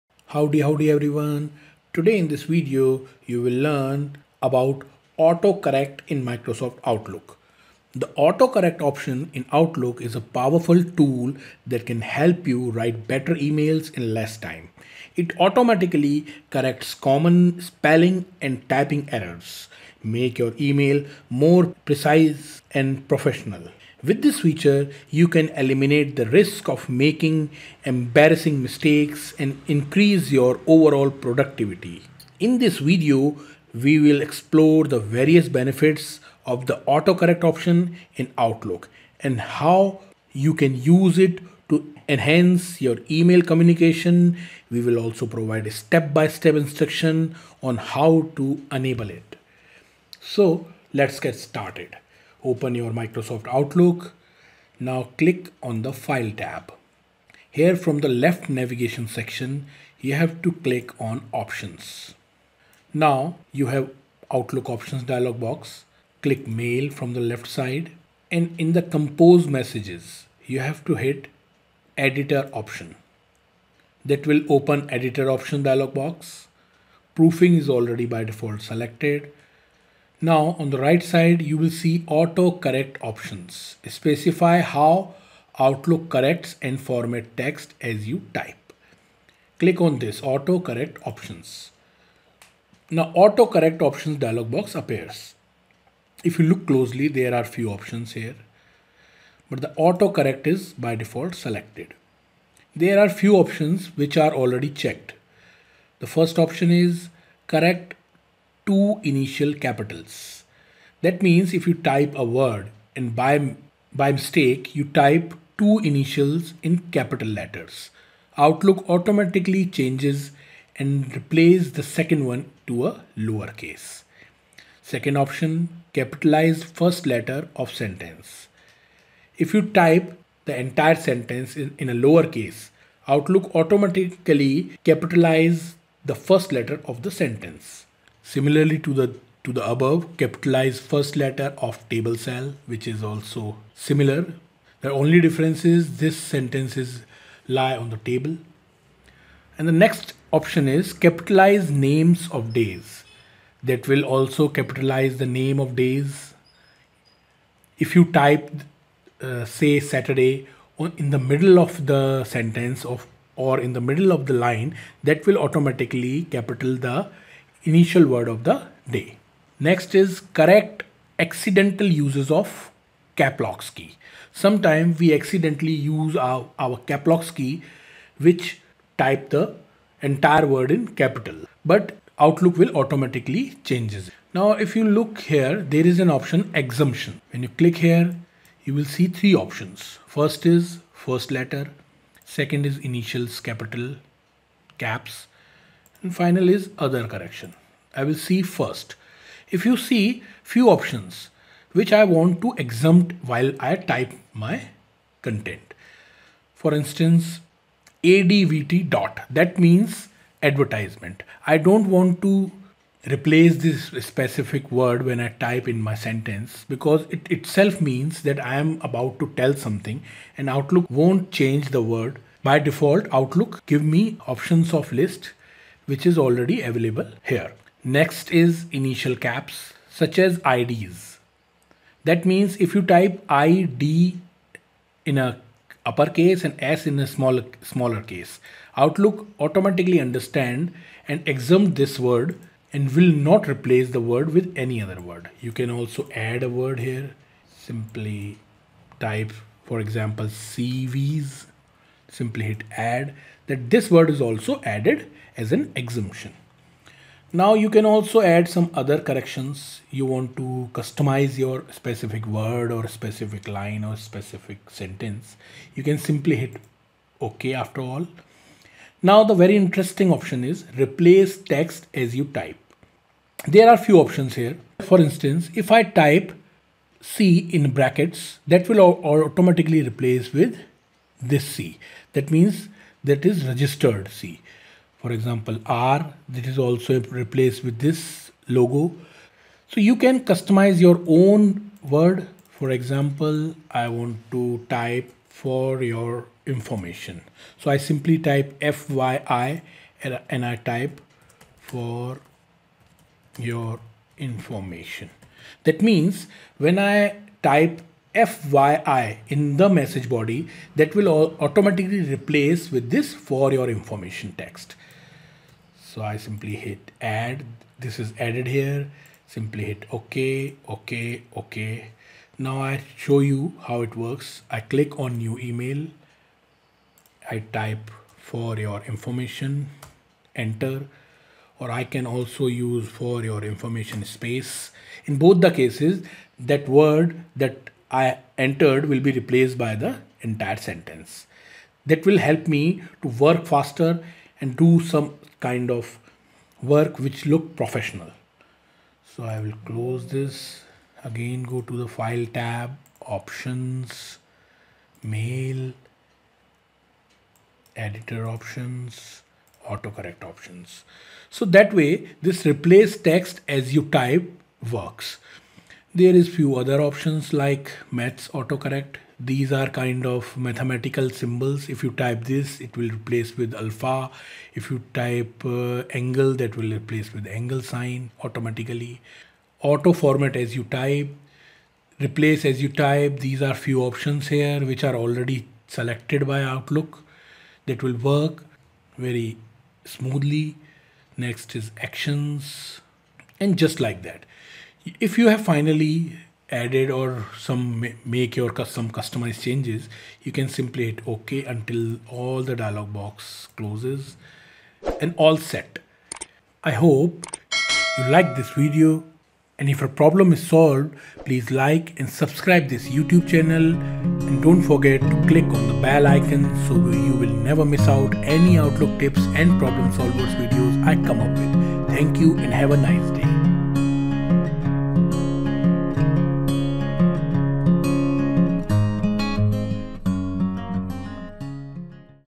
How to Use the AutoCorrect Feature in Outlook?
In today's fast-paced and ever-evolving work environment, efficient communication is key.

As emails continue to be the primary mode of formal communication in most organizations, it has become essential to be able to express ourselves effectively and efficiently.

However, typing errors and spelling mistakes can mar the message and create confusion.

That's where the autocorrect option in Microsoft Outlook comes in handy.

The autocorrect feature is not just limited to Word but can also be used in Outlook to save time and improve accuracy.

The autocorrect option in Outlook is a powerful tool that can help you write better emails in less time.

It automatically corrects common spelling and typing errors, making your emails more precise and professional.

With this feature, you can eliminate the risk of making embarrassing mistakes and increase your overall productivity.

In this video, we will explore the various benefits of the autocorrect option in Outlook and how you can use it to enhance your email communication. We will also provide step-by-step instructions on how to enable.

- EQUIPMENT USED
○ My camera